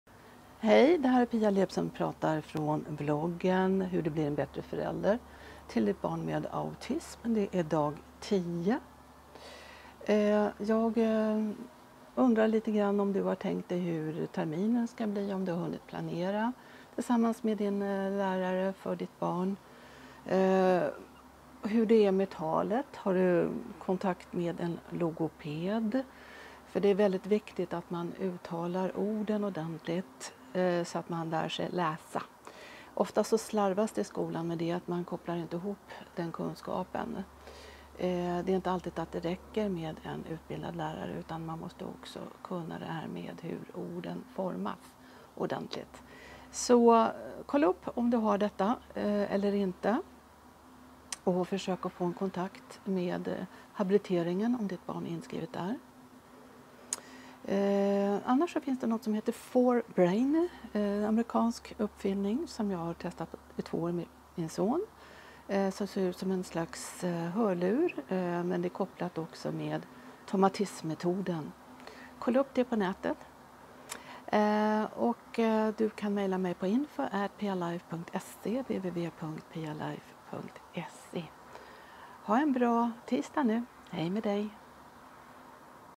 Vlog 10 Hur blir du en bättre förälder till ditt barn med autism? Kontakta en logoped.